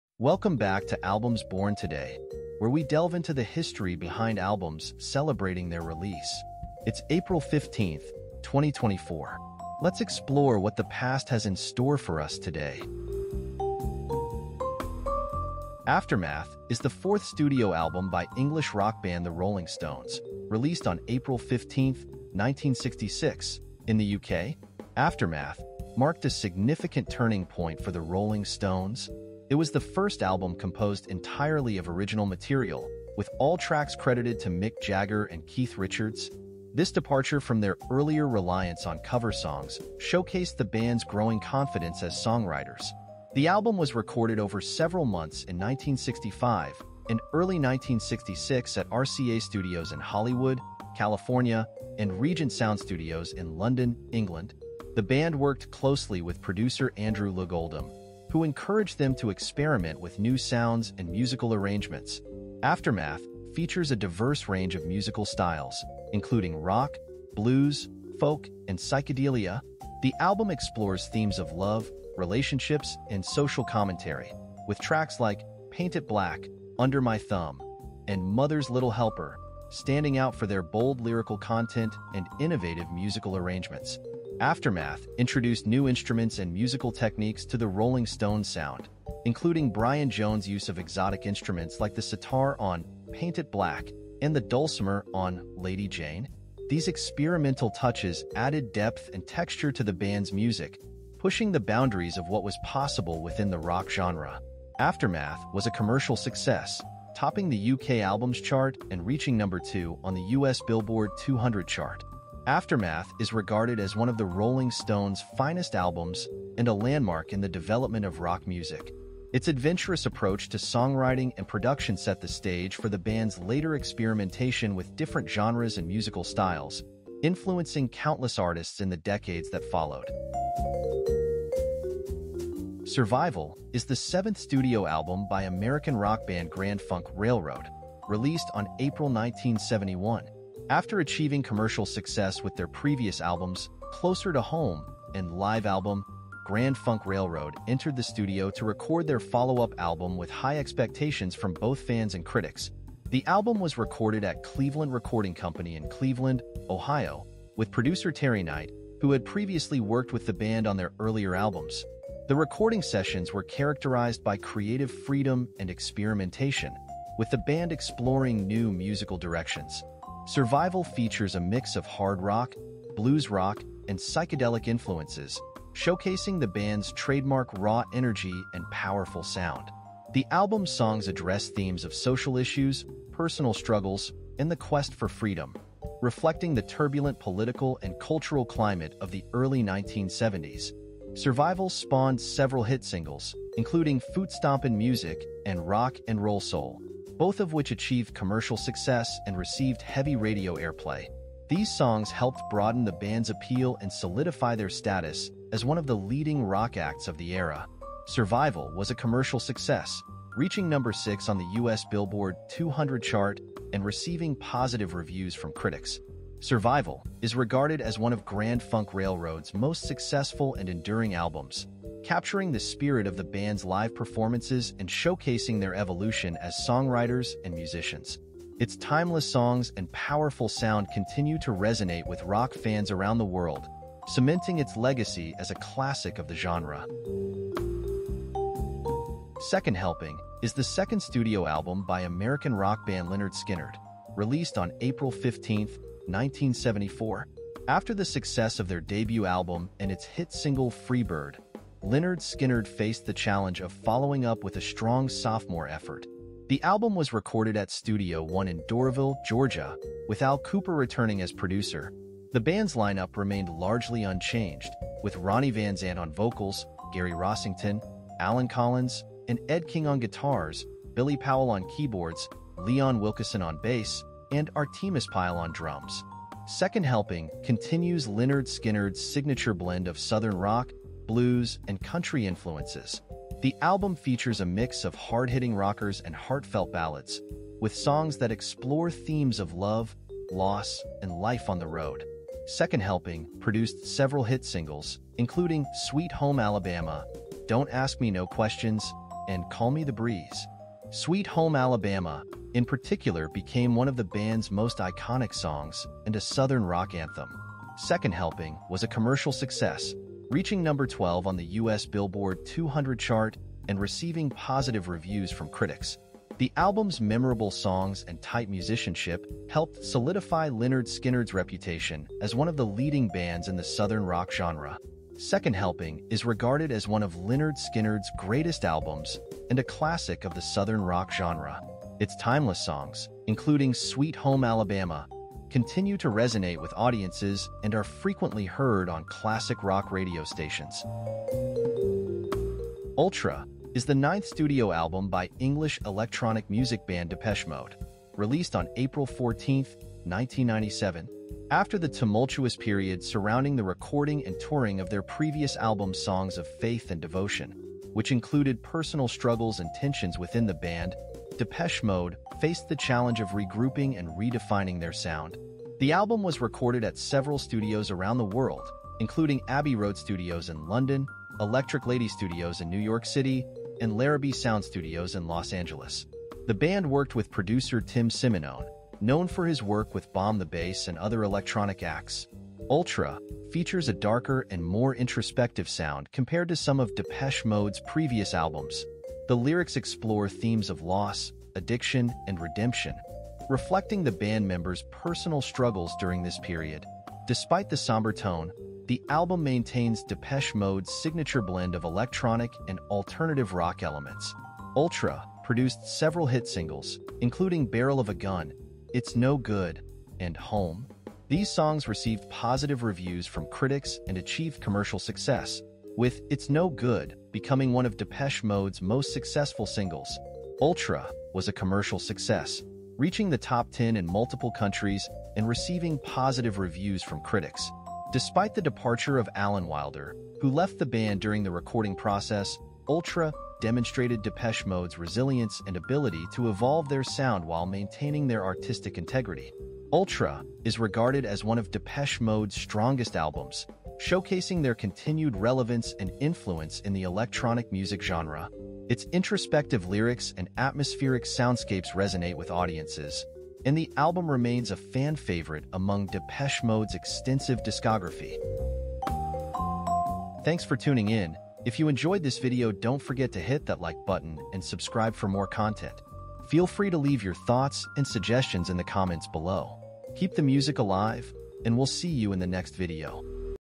On This Day: The Stories of Albums Released in the Past! 04/15/2024
Welcome back to Albums Born Today!
Step into the musical time machine with us as we embark on a journey through the annals of music history. In this video, we delve into the rich tapestry of albums released on this day in years past, uncovering the stories, the secrets, and the sheer brilliance behind each timeless masterpiece.